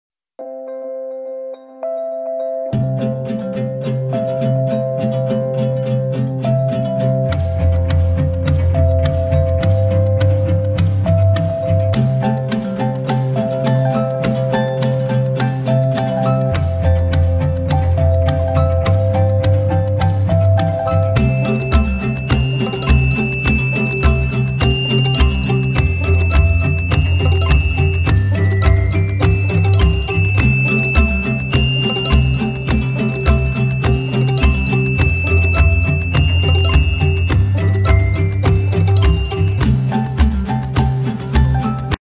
Mail's here! Mavic Air 2 drone flying higher than an airplane.
Watching a FedEx plane land during the sunset.